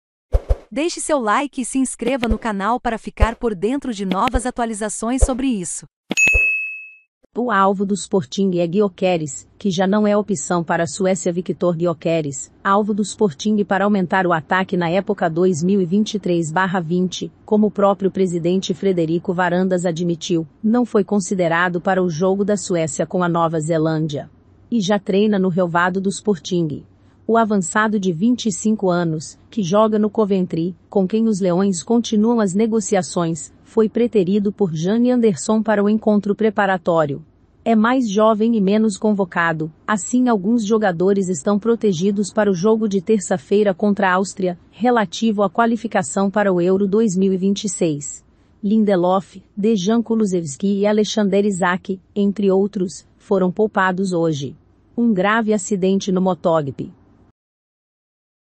O ALVO DO SPORTING É GYOKERES. QUE JÁ NÃO É OPÇÃO PARA A SUÉCIA
RelatedSport. St. Juste está de férias a preparar a época 2023/24 e já treina no relvado do Sporting.